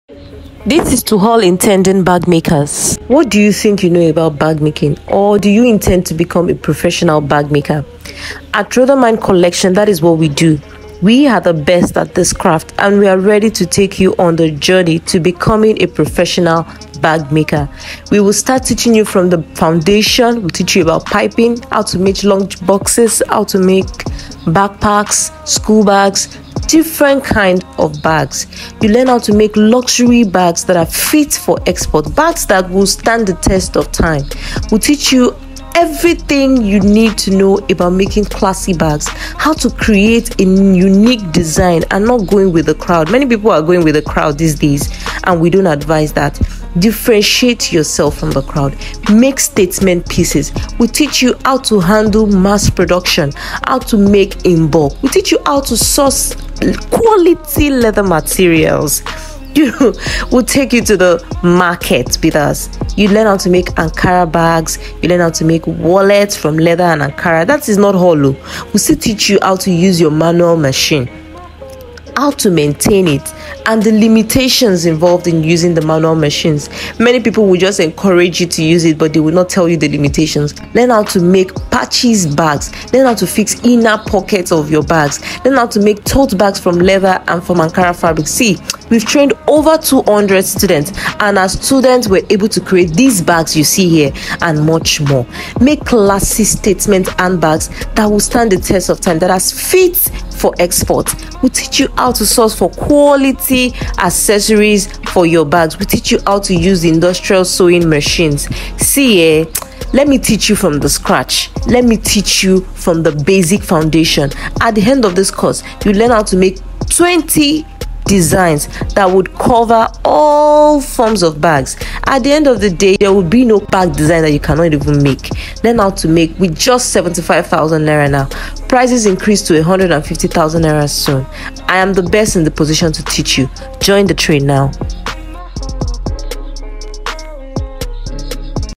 Enrol for our Online bag making course
This online course is what you need to scale this year . Many people have been searching for the right place to learn how to make bags online . We have different physical training packages but we’ve decided to bring it online for the benefit of those that are really far away and can’t make it to our atelier . This is your chance to learn all you need to know about professional bag making. Enrol for our Online bag making course now .

Get our early bird discount when you take action now we also have instalmental payment plan where you can pay twice. Send a dm for that